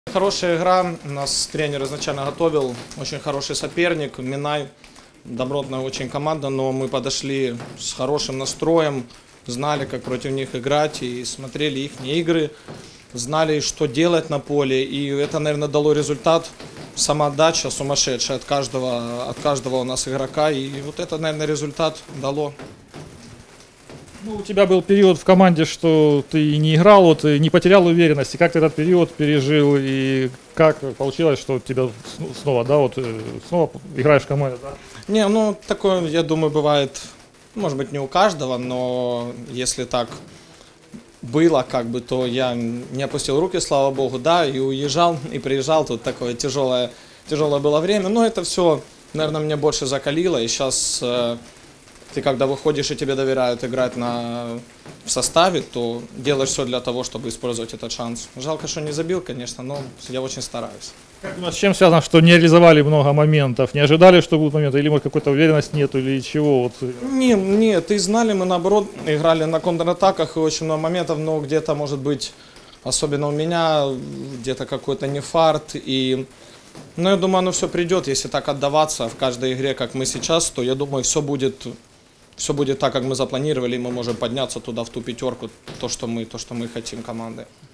Артем Козлов: "Очень хотелось забить гол"
Форвард Горняка-Спорт о выигранном матче с "Минаем" (1:0).